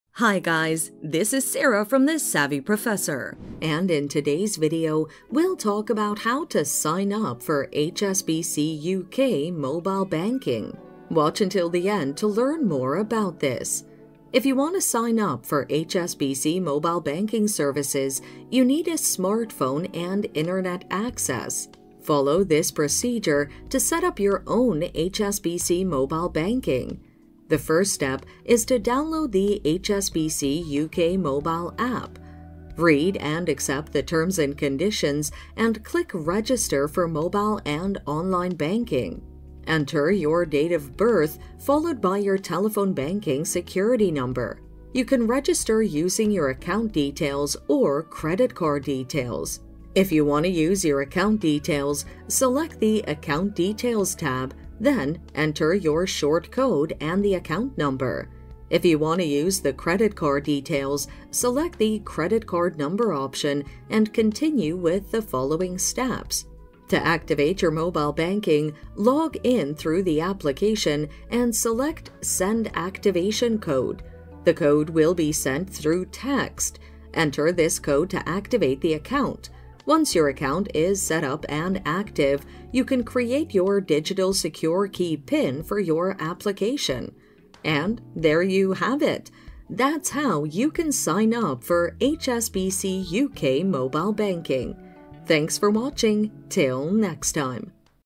How To Sign Up For HSBC UK Mobile Banking (How To Register For HSBC UK Mobile Banking)
How To Sign Up For HSBC UK Mobile Banking (How To Register For HSBC UK Mobile Banking). In this video tutorial I will show you how to sign up for HSBC UK mobile banking.

If you want to sign up for HSBC mobile banking services, you need a smartphone and internet access. Follow this procedure to set up your own HSBC mobile banking.
1. The first step is to download the HSBC UK mobile app.
3. Enter your date of birth followed by your telephone banking security number.
4. You can register using your account details or credit card details.
5. If you want to use your account details, select the "Account Details" tab, then enter your short code and account number.
6. If you want to use the credit card details, select the "Credit Card Number" option and continue with the following steps.

To activate your mobile banking, log in through the application and select 'Send activation code.' The code will be sent through text. Enter this code to activate the account. Once your account is set up and active, you can create your digital secure key PIN for your application.